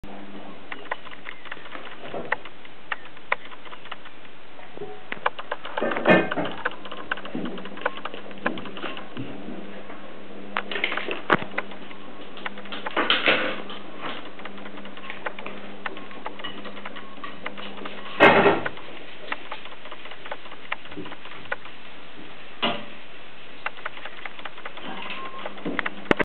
Krejzol kontra pralka
Jak widać, KOT lubi pralkę.